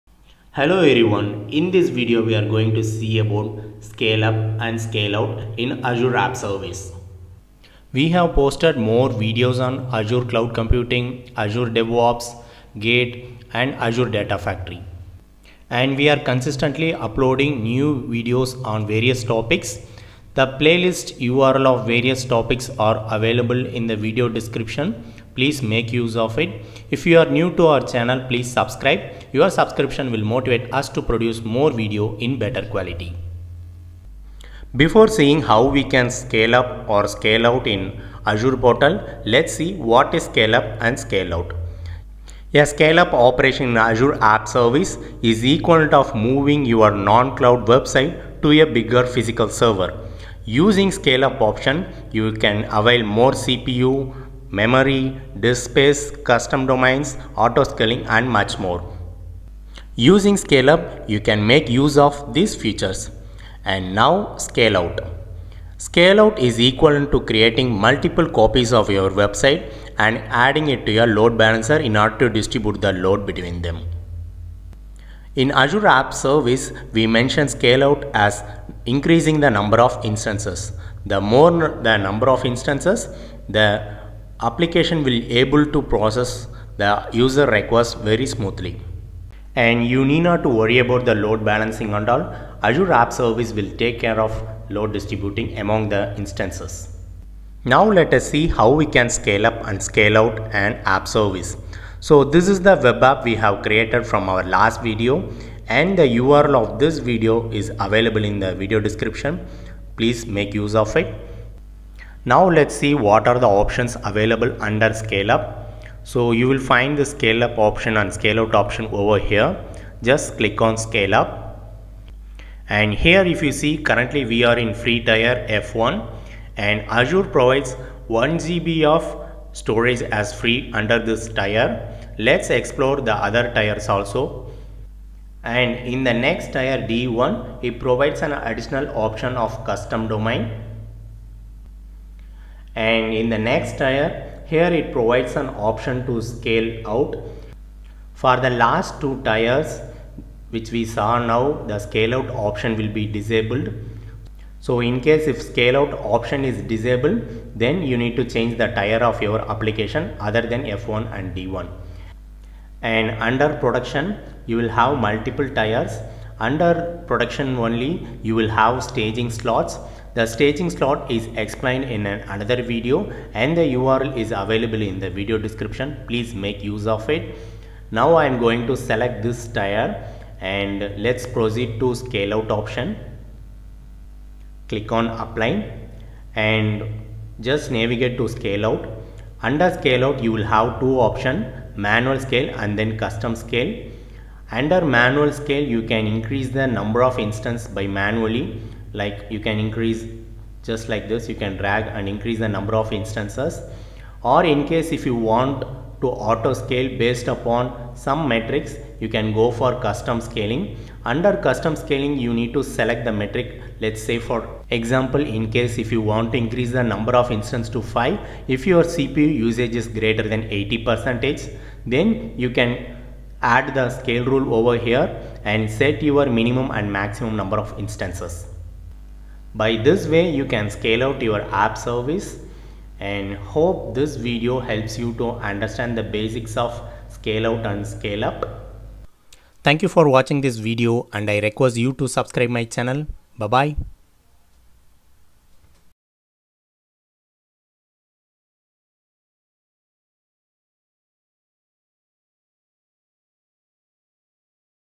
Scale up and Scale out in Azure App Service Explained
This video provides explanation on Azure App service scale up and scale out and we have showed how to scale up and scale out an Azure app service.